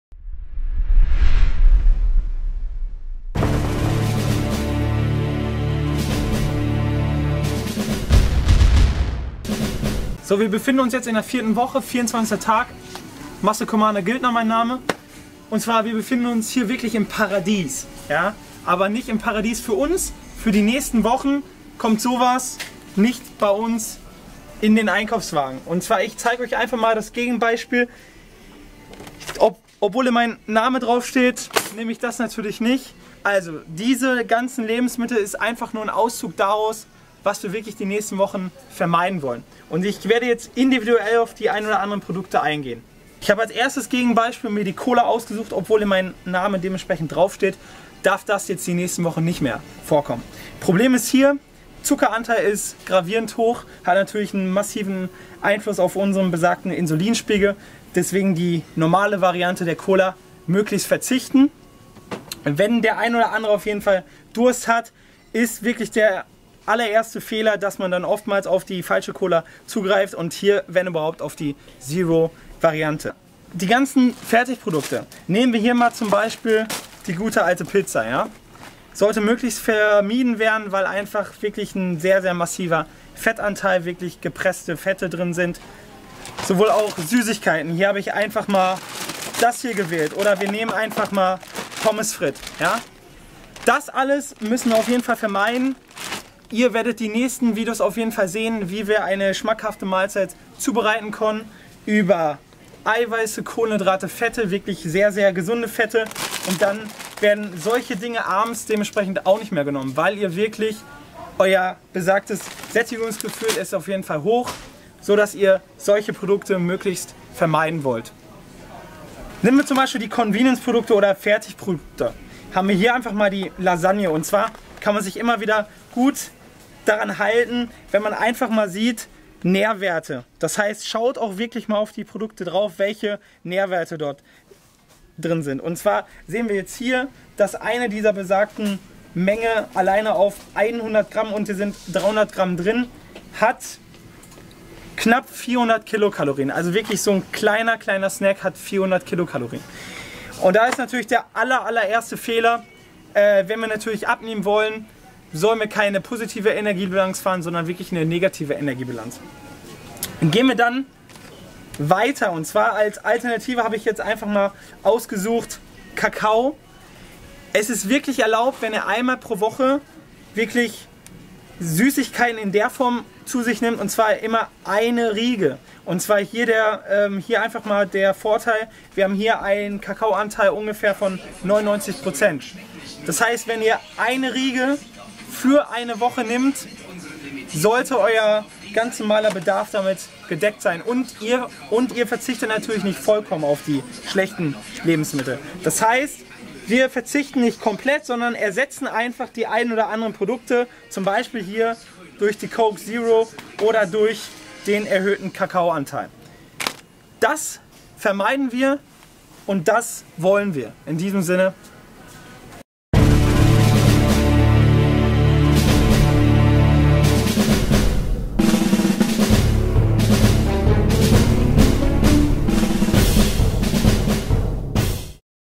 Daniel Gildner MUSCLE COMMANDER - Tag 24: No Cheat Days!
Tag 24: Regeneration

No Cheat Days!

Alkohol, Fast-Food, Softdrinks und Süßigkeiten sind für dich in den nächsten Wochen tabu. Alkohol bremst den Muskelaufbau und zerstört so deine Trainingsfortschritte. Fast-Food, Softdrinks und Süßigkeiten musst du aus deiner Ernährung streichen, weil Sie kurzkettige Kohlenhydrate beinhalten. Diese schnell verdaulichen Kohlenhydrate finden sich schneller auf deinen Hüften wieder als du „Muscle Commander" sagen kannst -- und das wollen wir ja nicht! Also Finger weg!

- Dein Shop für Sportlernahrung!